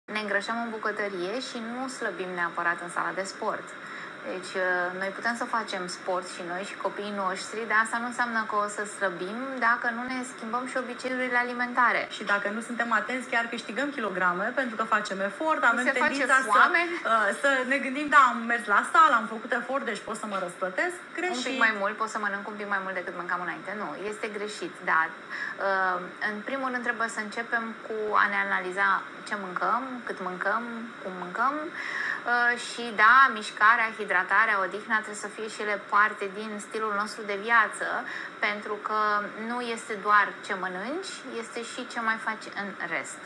Alege un stil de viata sanatos
Despre planificarea meselor, gustarilor, a pachetului pentru scoala sau pentru birou. Pe larg despre cum sustinem dezvoltarea copiilor si succesul lor la scoala cu Simona Balanescu.